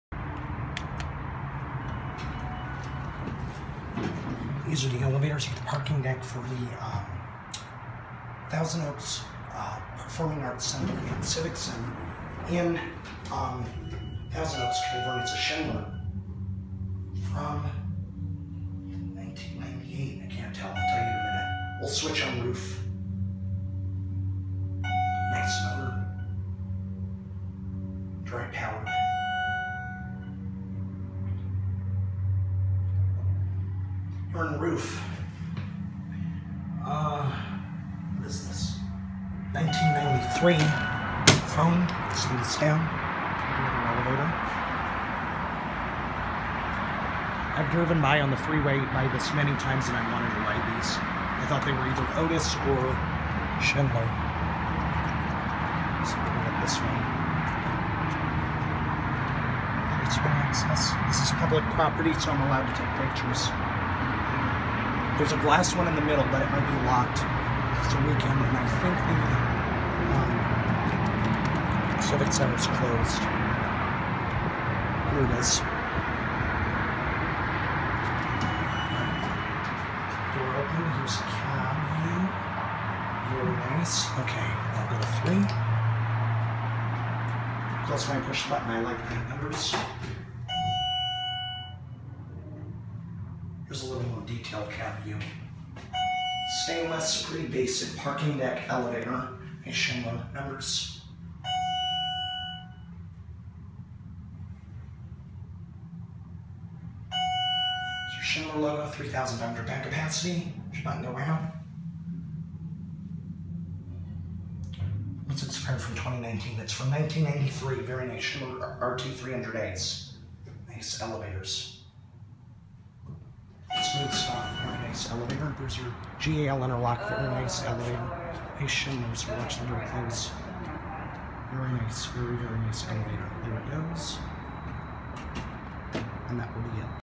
1993 Schindler 300A RT Hydraulic Parking Elevators @ City Hall, Thousand Oaks, CA
Pretty nice set of elevators!!! I could not access the main ones unfortunately. Installed in 1993.